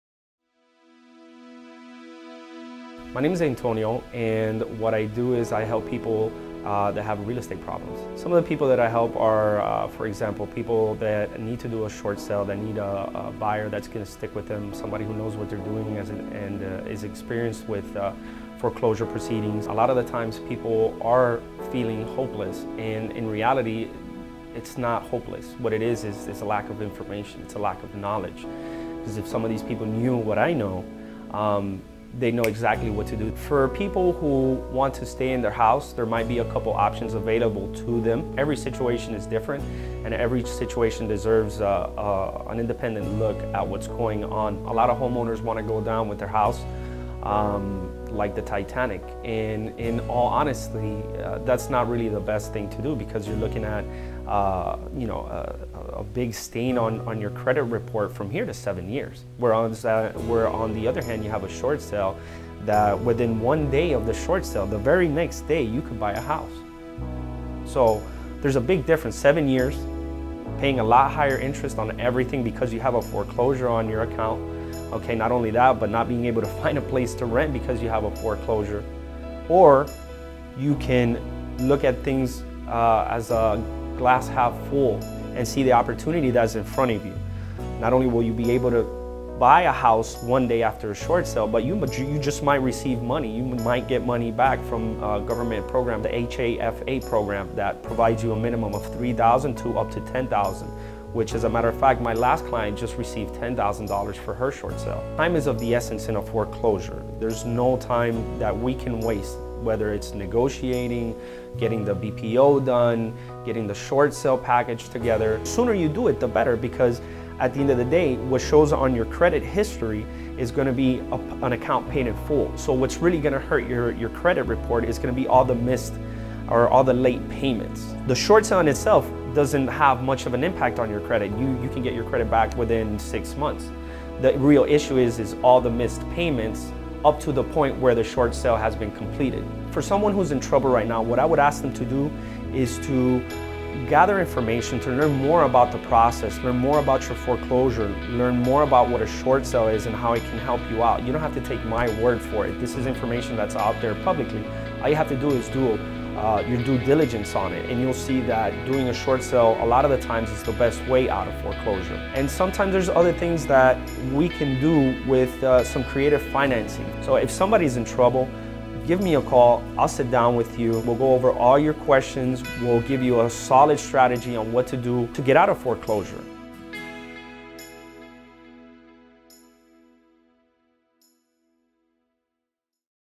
Short Sale Expert Full Interview | Miami Dade | Antonio Lopez | Antlop Miami
Miami Dade Short Sale expert, Antonio Lopez full interview at main office located in hometown of Miami Lakes. He's a short sale specialist found in Miami Dade county and can short sell your house successfully. Get to know Antonio Lopez, watch his interview.